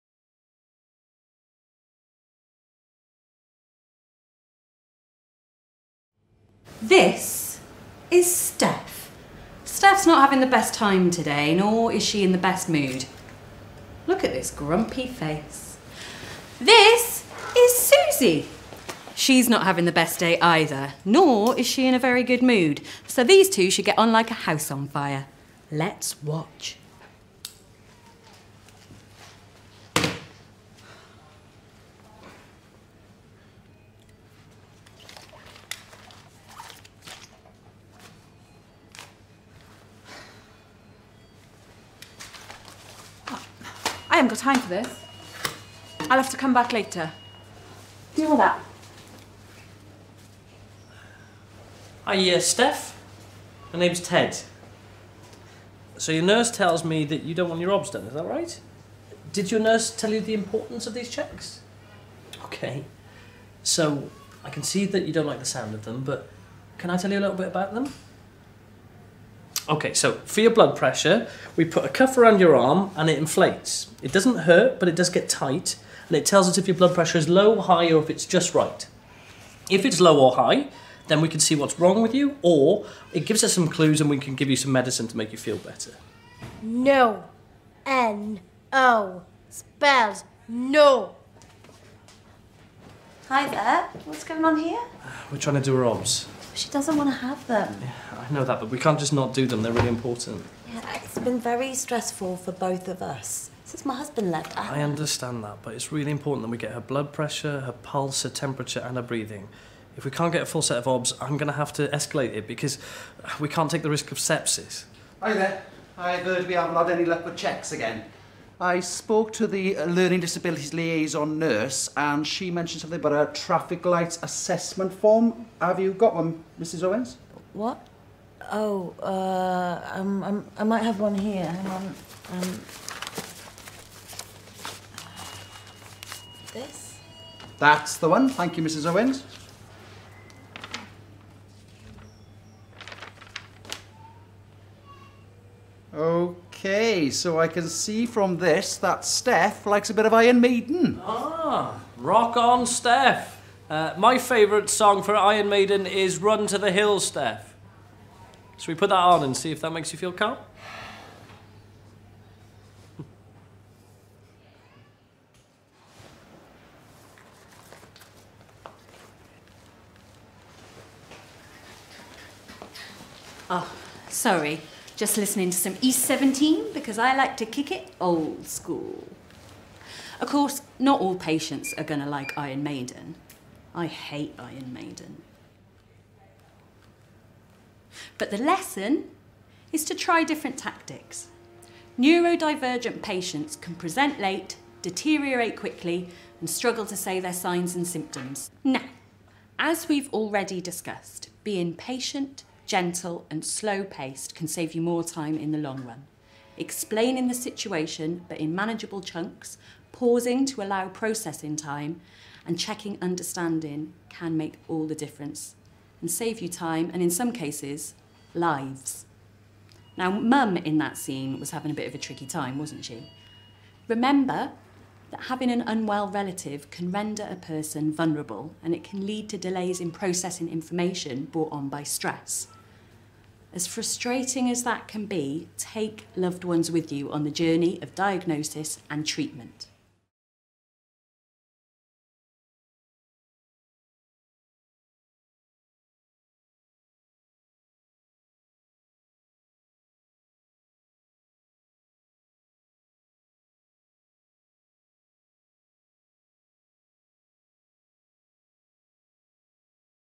How to..record observations, detect and respond to sepsis or deterioration when caring for a patient
How to..record observations, detect and respond to sepsis or deterioration when caring for an uncooperative patient